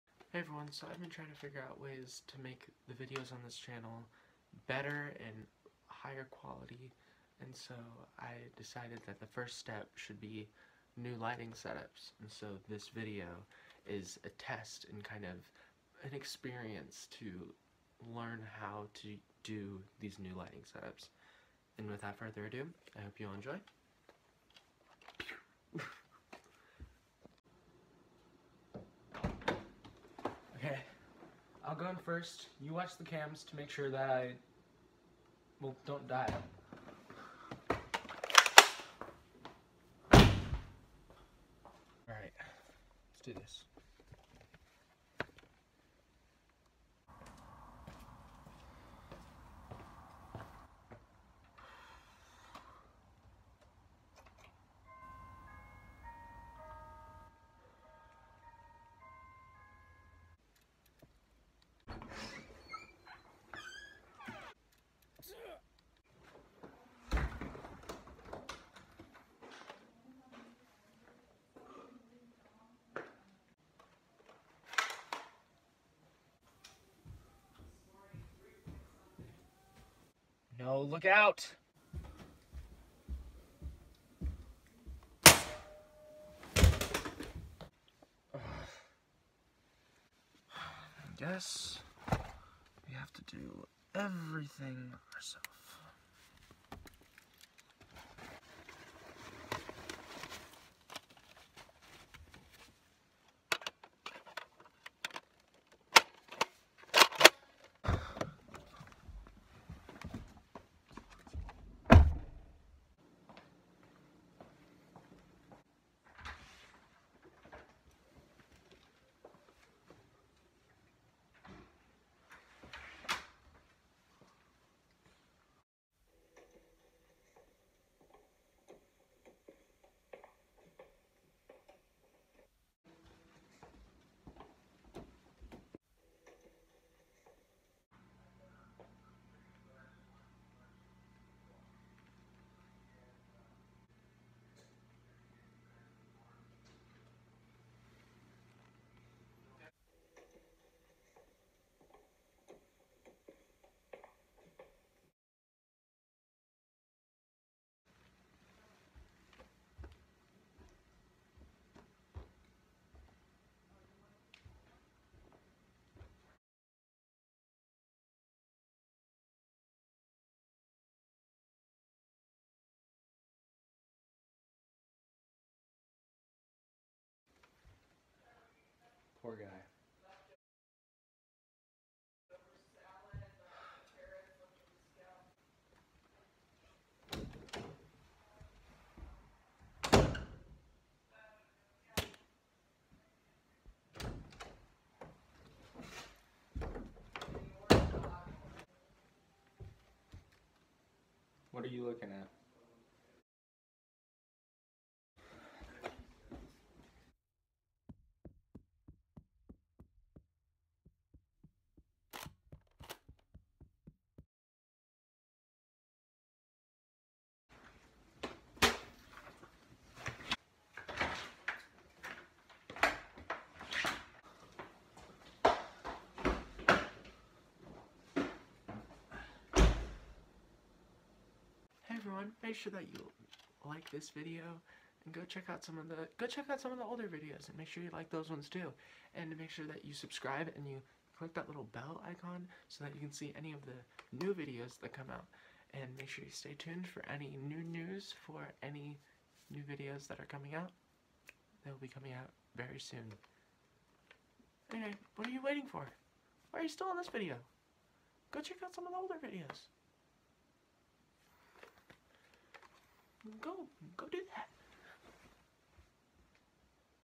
Double Trouble
Loosely based on Corridor’s SYNC, this short film is a test for new lighting techniques and setups.